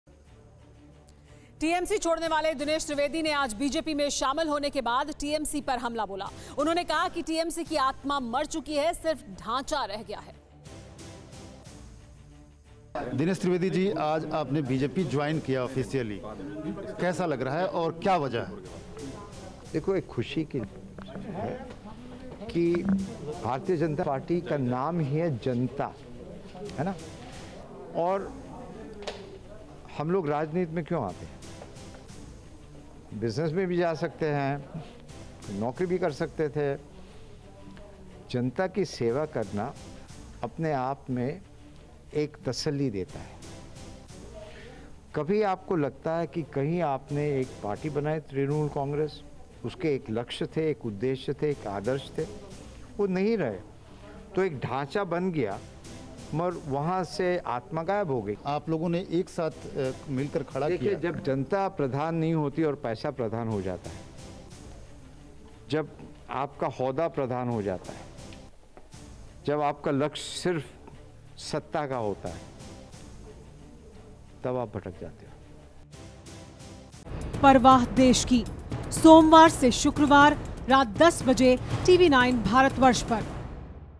BJP में शामिल हुए Dinesh Trivedi.. मुश्किल में पड़ी Mamata Banerjee
टीएमसी छोड़ने वाले दिनेश त्रिवेदी ने आज बीजेपी में शामिल होने के बाद टीएमसी पर हमला बोला. उन्होंने कहा कि टीएमसी की आत्मा मर चुकी है.. सिर्फ़ ढांचा रह गया है.
Credit